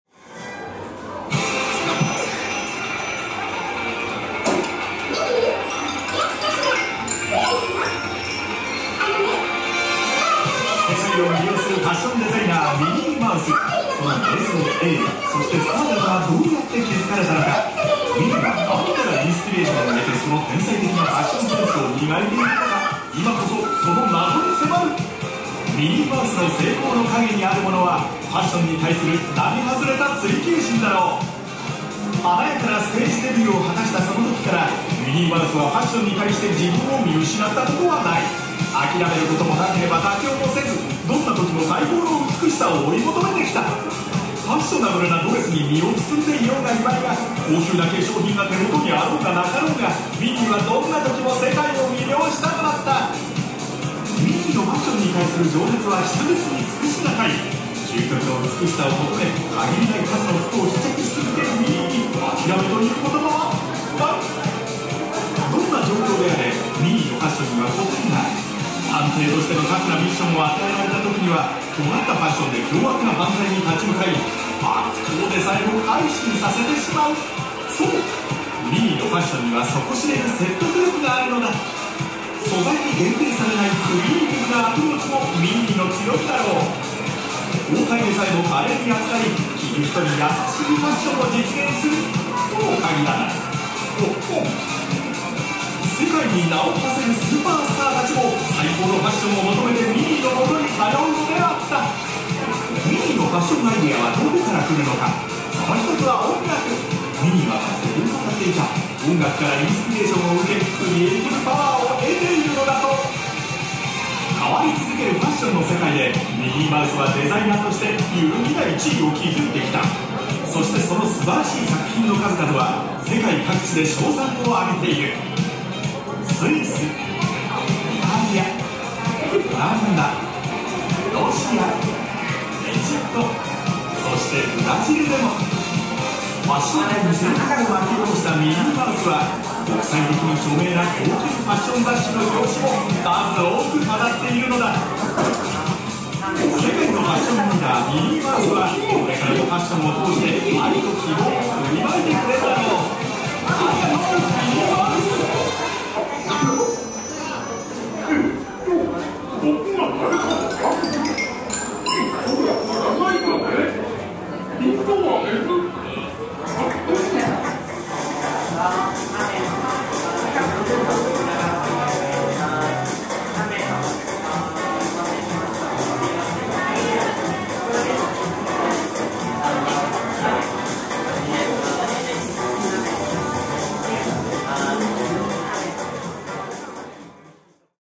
ミニスタにあるテレビの動画・全編（2022年5月15日 TDL）
なかなかじっくりと見ることのできなかった、ミニスタの入り口に設置されているテレビ。近頃のミニスタは室内にはいると、大量に生地ロールの置かれてる部屋までは自由に行動ができますので、じっくりと撮ってきました。